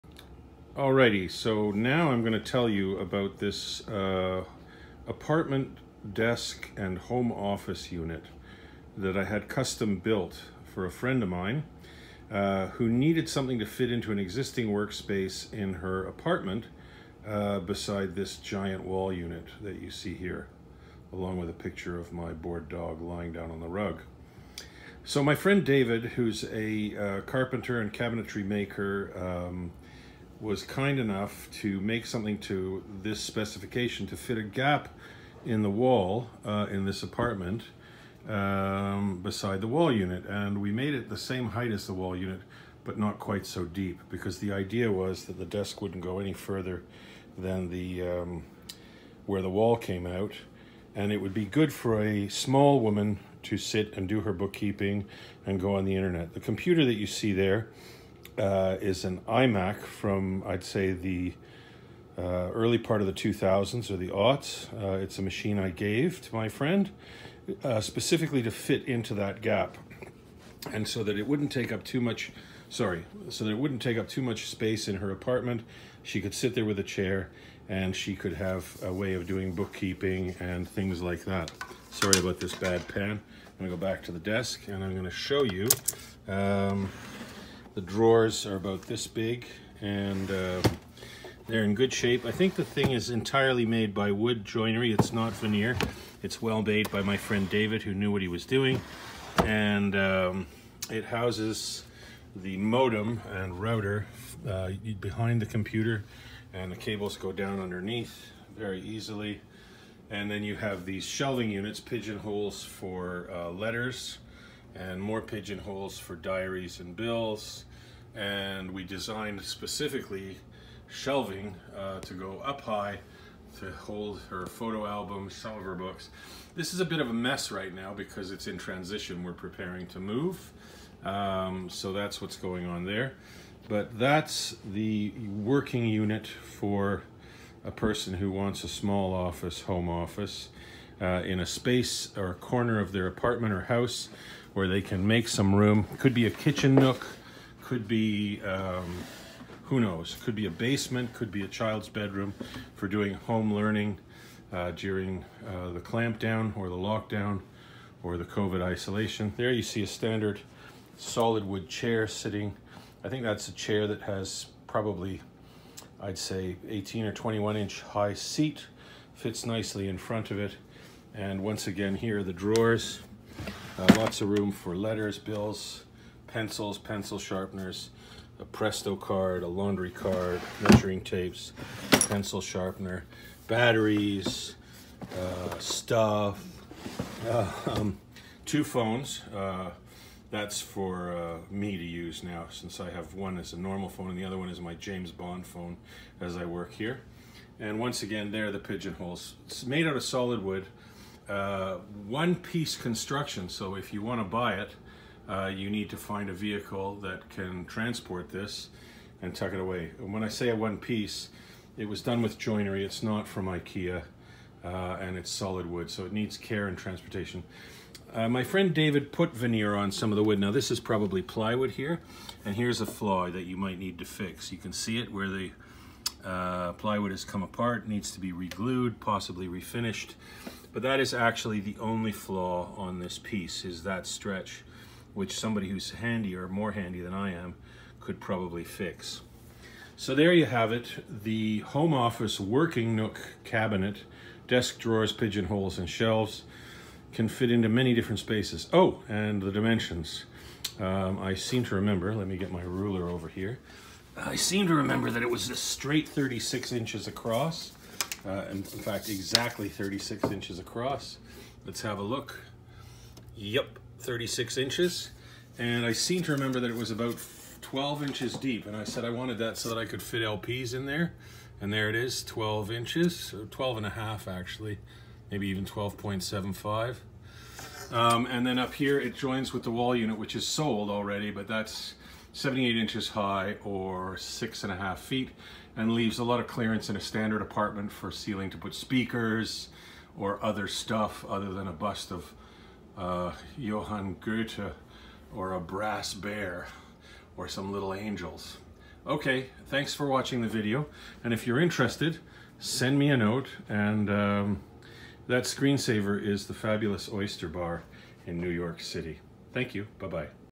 Sweet Little Home Office Working Nook Desk
One correction to the video. The unit is a combination of solid wood pieces along with some plywood elements that are finished with veneer and stain. Structurally very sound, and needs the minor repair that is noted in the video.

Dimensions are 36” W x 12.5” D x 78” H